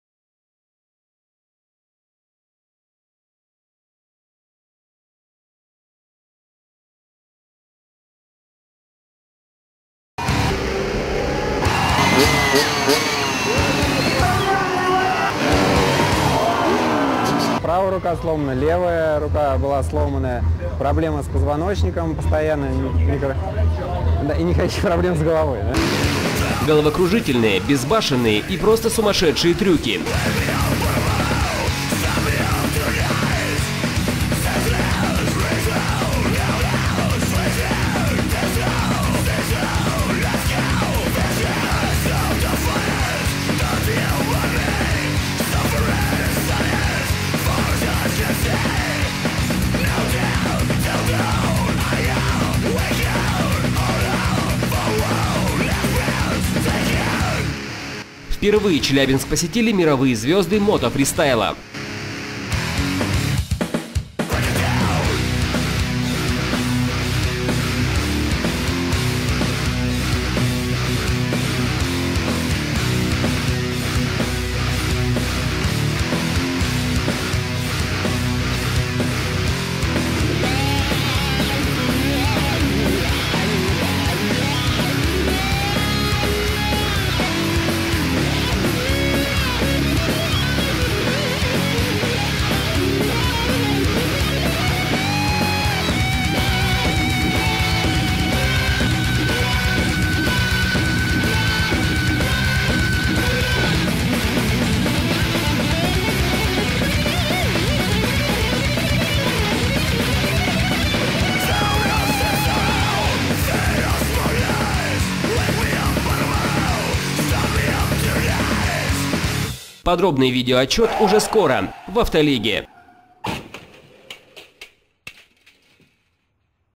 Adrenalin FMX Rush 2011 Челябинск
Ведущие мировые звёзды мотоспорта покажут себя в четырех дисциплинах: стантрайдинг, BMX, маунтинбайк (MTB) и мотофристайл (FMX)